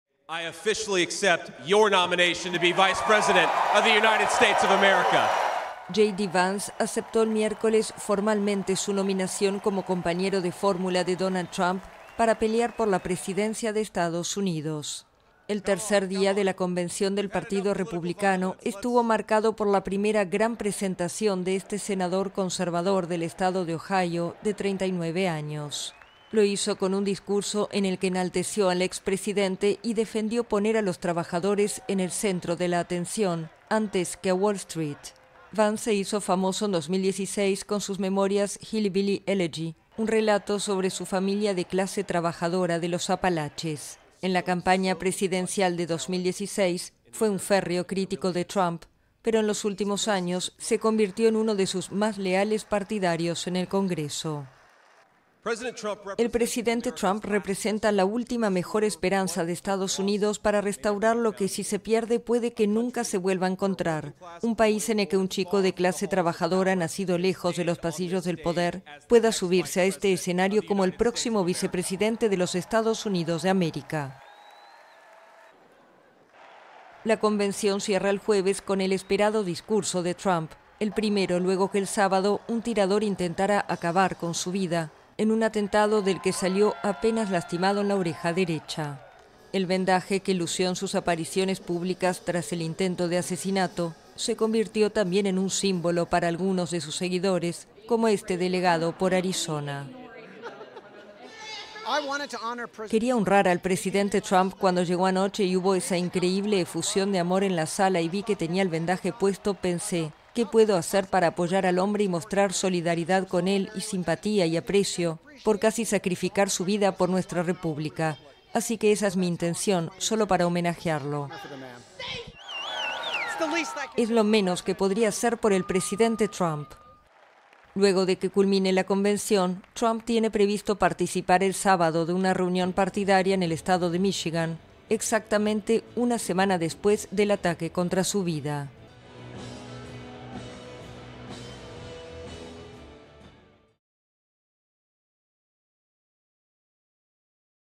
Compañero de fórmula Republicano, J.D. Vance, enaltece a Trump en su presentación oficial
El tercer día de la convención del partido republicano en Estados Unidos estuvo marcado por la primera gran presentación de J.D. Vance, un senador conservador del estado de Ohio elegido por Donald Trump para ser su compañero de fórmula.